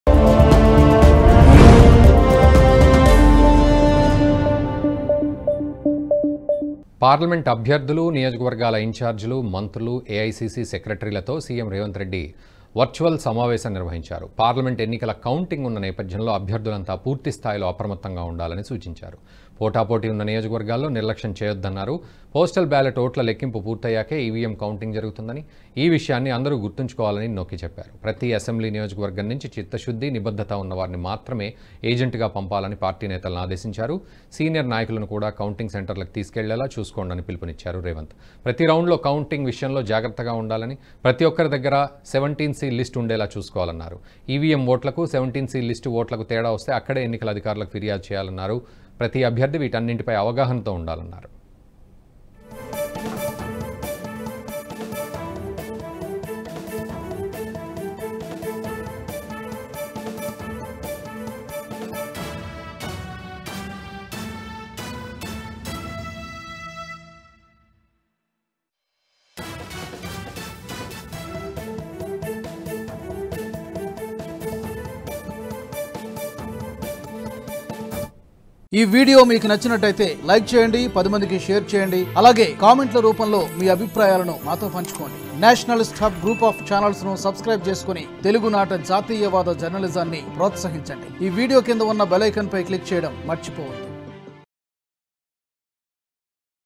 CM Revanth Reddy Speech About Parliament Election Counting | Congress | Telangana News | NHTV
Our Company account details :
Name : DHARMAGNYA BROADCASTING PVT LTD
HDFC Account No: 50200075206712
IFSC Code: HDFC0008045